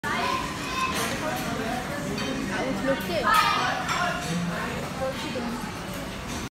Delicious pani puri #youtube
Gup chup call pani puri this is Indian street food type of chaat